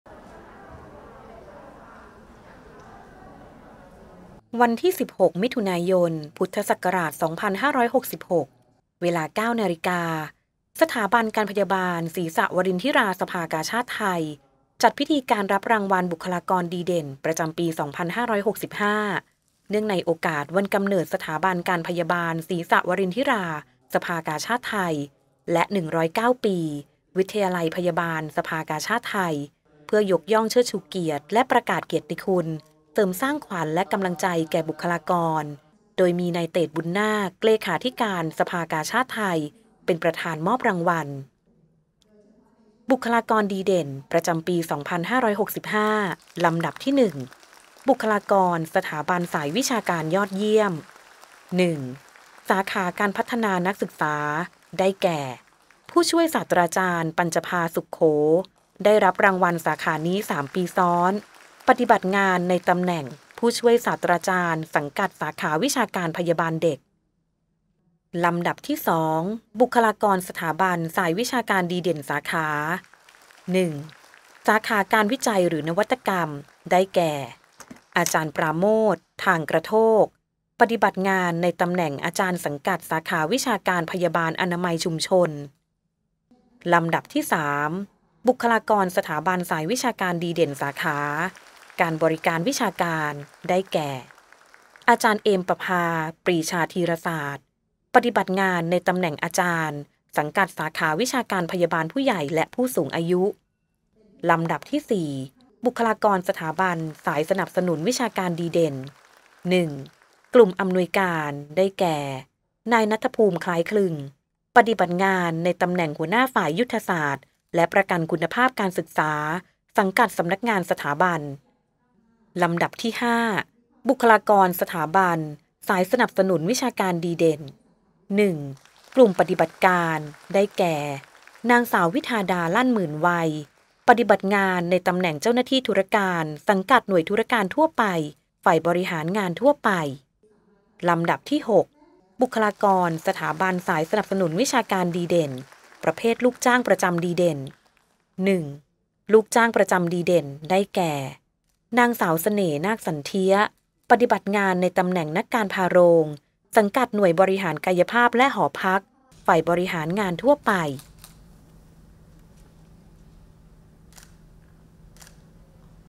สถาบันการพยาบาลศรีสวรินทิรา สภากาชาดไทย จัดพิธีการรับรางวัลบุคลากรดีเด่นประจำปี 2565
สถาบันการพยาบาลศรีสวรินทิรา สภากาชาดไทย จัดพิธีการรับรางวัลบุคลากรดีเด่นประจำปี 2565 เนื่องในโอกาสวันกำเนิดสถาบันการพยาบาลศรีสวรินทิรา  สภากาชาดไทย และ 109 ปี วิทยาลัยพยาบาลสภากาชาดไทย เพื่อยกย่องเชิดชูเกียรติและประกาศเกียรติคุณ เสริมสร้างขวัญและกำลังใจแก่บุคลากร โดยมีนายเตช บุนนาค เลขาธิการสภากาชาดไทย เป็นประธานมอบรางวัล เมื่อวันที่ 16 มิถุนายน 2566

โดยบุคลากรดีเด่น ประจำปี 2565 มีรายนามดังต่อไปนี้
ลำดับที่ 1 บุคลากรสถาบันสายวิชาการยอดเยี่ยม
สาขา การพัฒนานักศึกษา  ได้แก่ ผู้ช่วยศาสตราจารย์ปัญจพาณ์ สุขโข ได้รับรางวัลสาขานี้ 3 ปีซ้อน
ปฏิบัติงานในตำแหน่ง ผู้ช่วยศาสตราจารย์ สังกัด สาขาวิชาการพยาบาลเด็ก
ลำดับที่  2 บุคลากรสถาบันสายวิชาการดีเด่นสาขา
สาขา การวิจัยหรือนวัตกรรม ได้แก่    อาจารย์ปราโมทย์ ถ่างกระโทก
ปฏิบัติงานในตำแหน่ง อาจารย์ สังกัด สาขาวิชาการพยาบาลอนามัยชุมชน
ลำดับที่  3 บุคลากรสถาบันสายวิชาการดีเด่นสาขา การบริการวิชาการ  ได้แก่
อาจารย์เอมปภา ปรีชาธีรศาสตร์ ปฏิบัติงานในตำแหน่ง อาจารย์ สังกัด สาขาวิชาการพยาบาลผู้ใหญ่และผู้สูงอายุ
ลำดับที่ 4 บุคลากรสถาบันสายสนับสนุนวิชาการดีเด่น

กลุ่ม อำนวยการ  ได้แก่    นายณัฐภูมิ คล้ายคลึง
ปฏิบัติงานในตำแหน่ง หัวหน้าฝ่ายยุทธศาสตร์และประกันคุณภาพการศึกษา สังกัด สำนักงานสถาบัน
ลำดับที่ 5 บุคลากรสถาบันสายสนับสนุนวิชาการดีเด่น
กลุ่ม ปฎิบัติการ  ได้แก่    นางสาววิธาดา ลั่นหมื่นไวย์
ปฏิบัติงานในตำแหน่ง เจ้าหน้าที่ธุรการ สังกัด หน่วยธุรการทั่วไป ฝ่ายบริหารงานทั่วไป
ลำดับที่ 6 บุคลากรสถาบันสายสนับสนุนวิชาการดีเด่น

ประเภทลูกจ้างประจำดีเด่น
ลูกจ้างประจำดีเด่น  ได้แก่   นางสาวเสน่ห์ นาคสันเทียะ
ปฏิบัติงานในตำแหน่ง นักการภารโรง สังกัด หน่วยบริหารกายภาพและหอพัก ฝ่ายบริหารงานทั่วไป